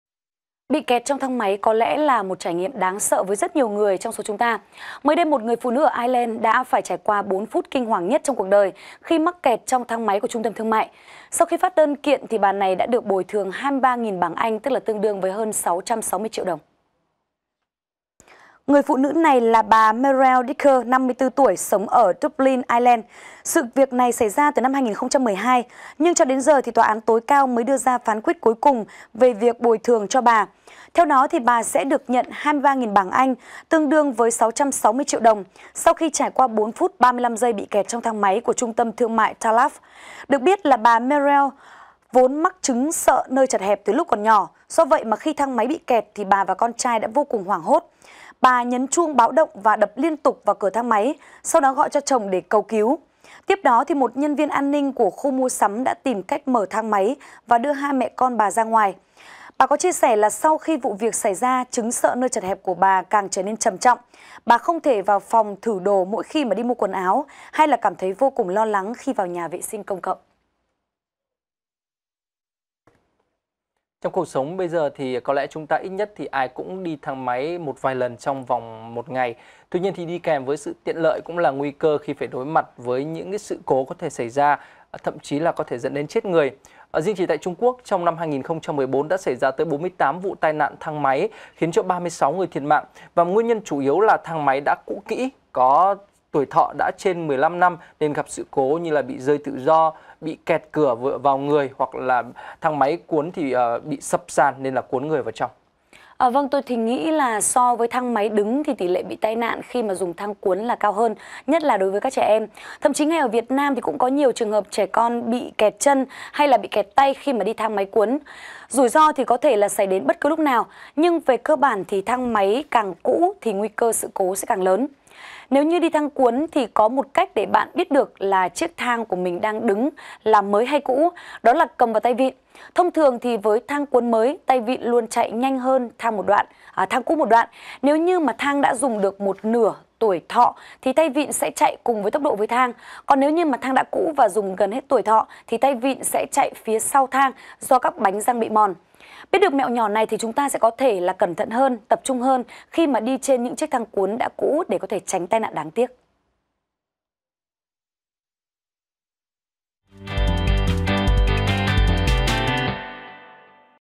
(VTC14)_Kẹt 4 phút trong thang máy, được bồi thường 660 triệu đồng
(VTC14) - Bị kẹt trong thang máy có lẽ là trải nghiệm đáng sợ với rất nhiều người trong chúng ta. Mới đây, một phụ nữ ở Ireland đã phải trải qua 4 phút kinh khủng nhất trong cuộc đời khi mắc kẹt trong thang máy của TTTM. Sau khi phát đơn kiện thì bà này đã được bồi thường  23.000 bảng Anh (tương đương hơn 660 triệu).